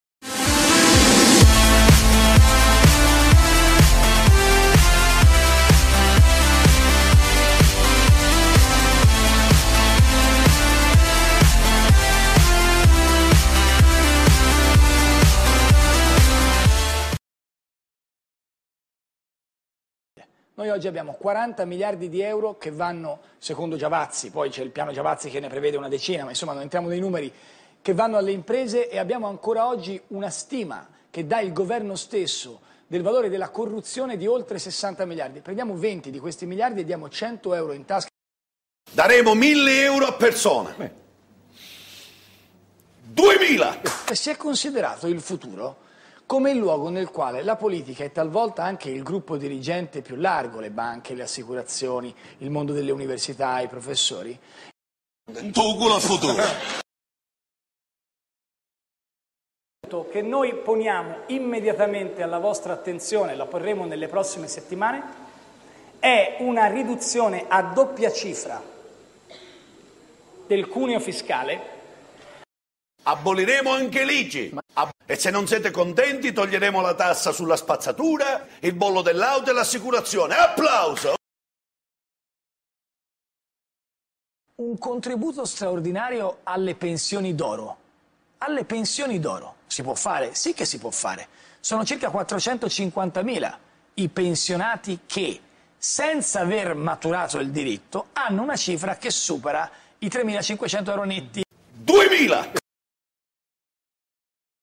Matteo Renzi Vs Cetto la Qualunque
mix di discorsi di Matteo Renzi fatti in varie trasmissioni, e confrontati a Cetto la Qualunque